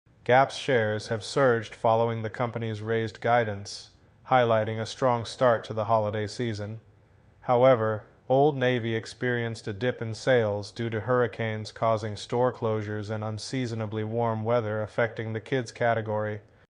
Gap Shares Rise Amid Holiday Optimism and Challenges
Gap has recently experienced a surge in its shares, fueled by an optimistic outlook for the holiday season. The company raised its guidance, reflecting a strong start to this critical retail period. However, not all of Gap's brands are thriving; Old Navy reported a decline in sales due to the adverse effects of hurricanes that led to store closures, along with warm weather that negatively impacted sales in its kids category. This juxtaposition highlights the complexities of the retail landscape as consumers head into the holiday shopping frenzy.

Daniel Aharonoff: Earnings Engage: The Business Pulse
Episode